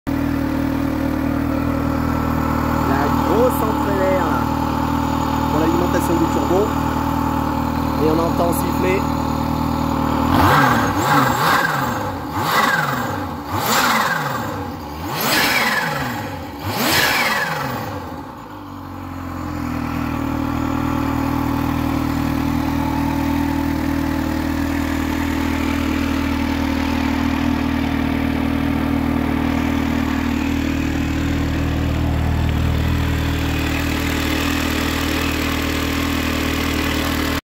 Z H2 SON DU COMPRESSEUR ....
Joli sifflement du compresseur ..
En attendant la vidéo de l'essai a venir ...je roule avec pendant 2 semaines et je vous transmet mon ressenti global.
Sensation garantie et combat a chaque courbe.